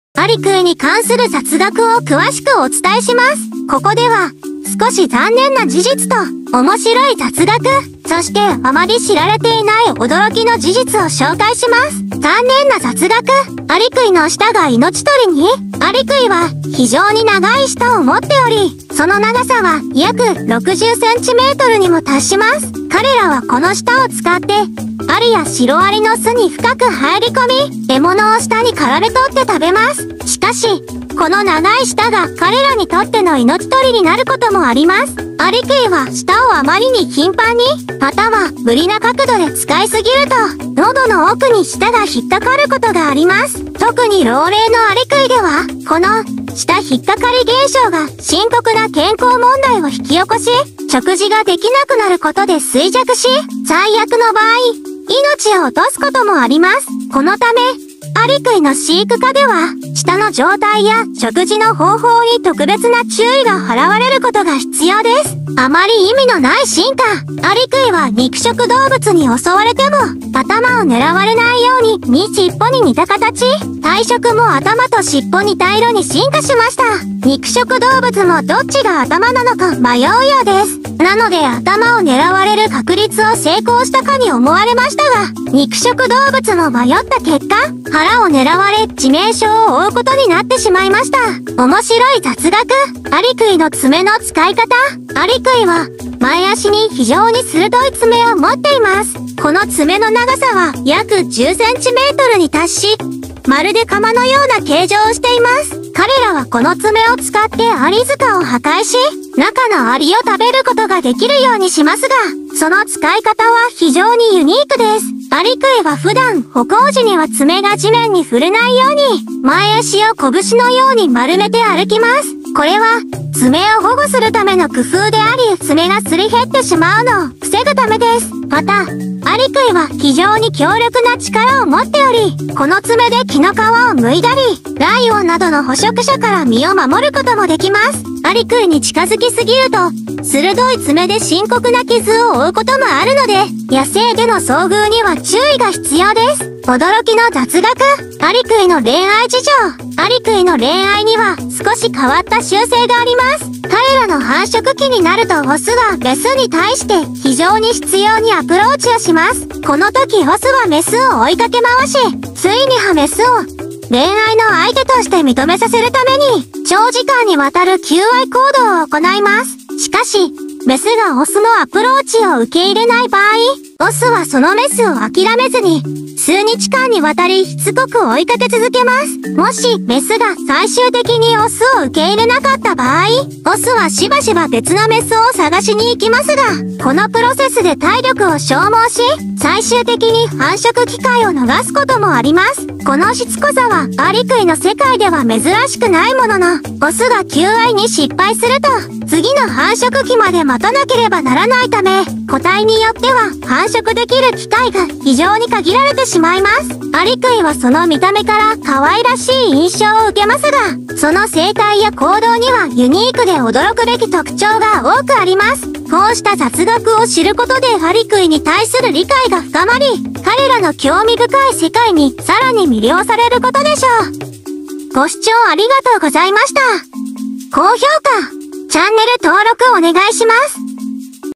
アリクイの残念な雑学＆面白い雑学
アリクイの魅力は、そのユニークな外見と特化した生態にあります。細長い鼻と舌を持ち、アリやシロアリを食べるために進化したアリクイは、自然界での役割が非常に特異です。また、長くてふわふわした尾や、ゆったりとした動きが可愛らしく、見ているだけで癒されます。静かで穏やかな性格もあり、観察することでその独自の魅力を感じることができます。